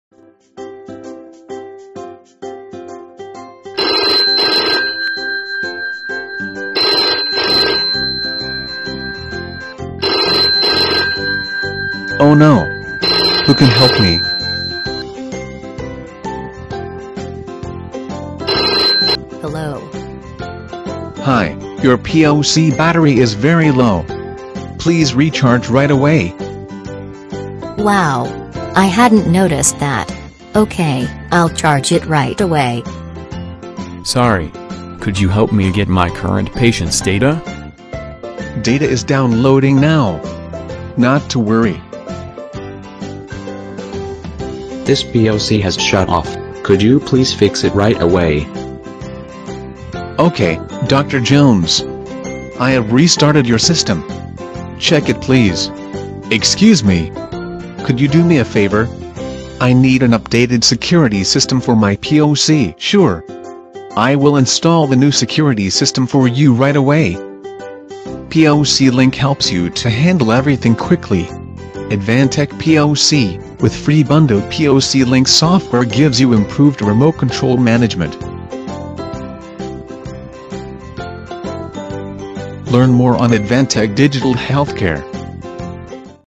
POC EP3 Remote Control, Advantech (EN)
ADVANTECH POC is widely used in critical area. With POC Link software,it's easy for you to control all the PC in your hospital.